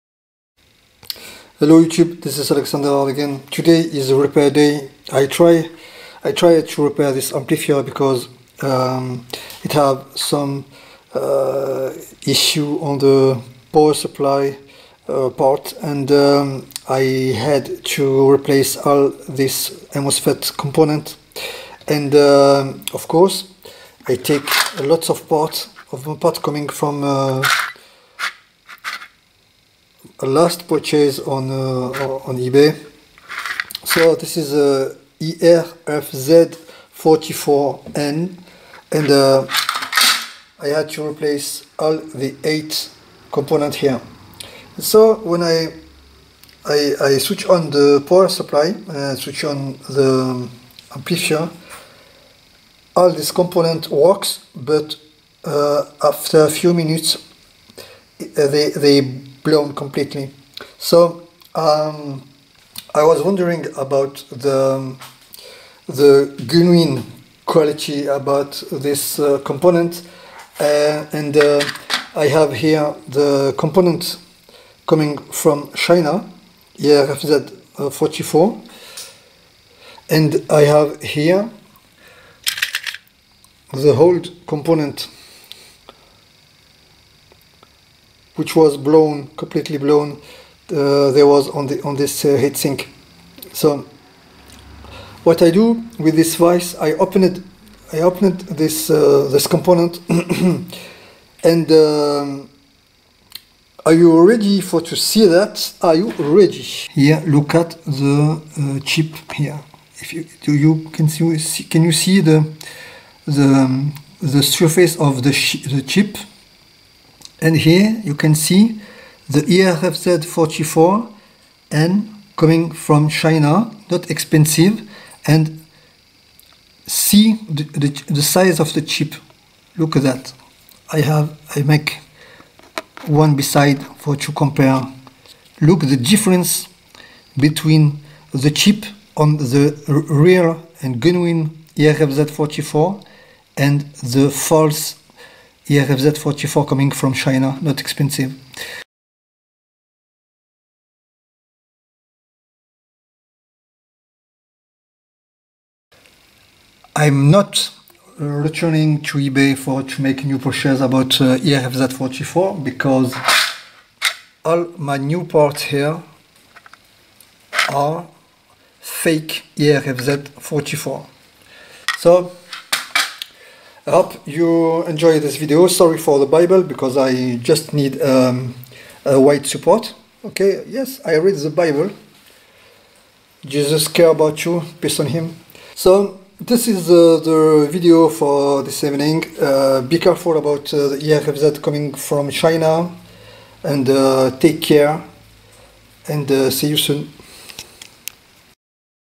MOSFET from China, IRFZ44, too cheap to be true
This case I can't make a repair. Cause my stock of IRFZ44 are all false.
SORRY FOR THE BIBLE... I was reading it and I was just needing a white support, I made this video very very quickly.
Dans ce cas je n'ai pas pu réparer l'ampli immédiatement. J'ai réussi à trouver de vieux transistors que j'avais aussi acheté en Chine, mais qui étaient vrais cette fois-ci, donc la réparation a pu être faite.